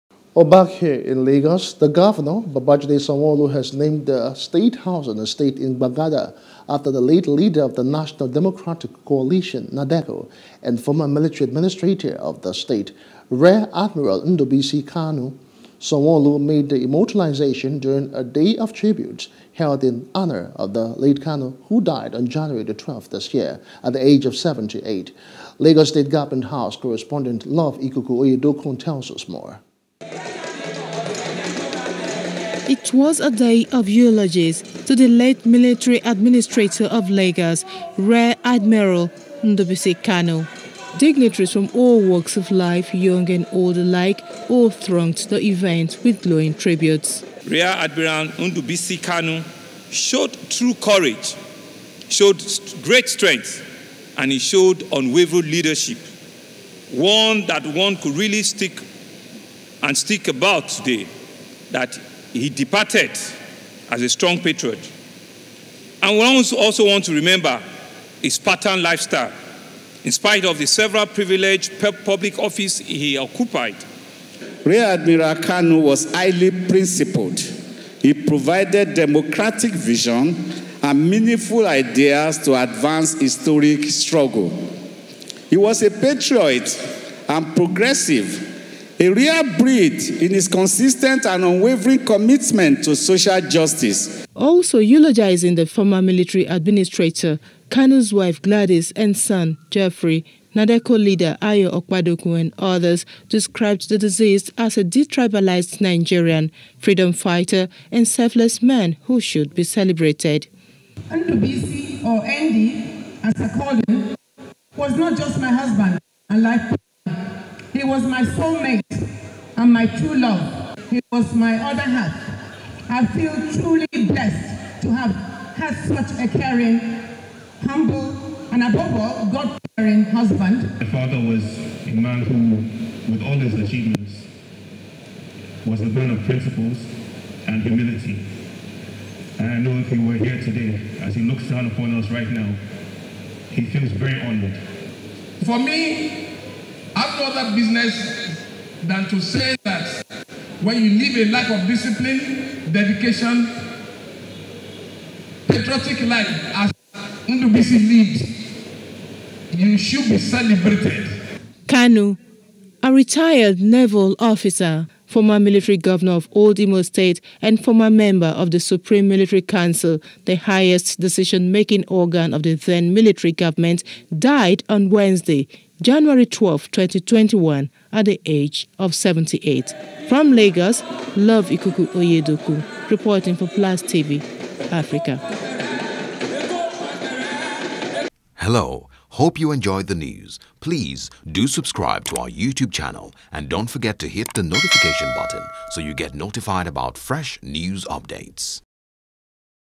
Sanwo Olu Names Housing Estate After Military Administrator | NEWS
Governor Babajide Sanwo-Olu has named the lagos state housing estate in gbagada after the late leader of the national democratic coalition, NADECO, and former military administrator of the state, rear admiral Ndubuisi Kanu.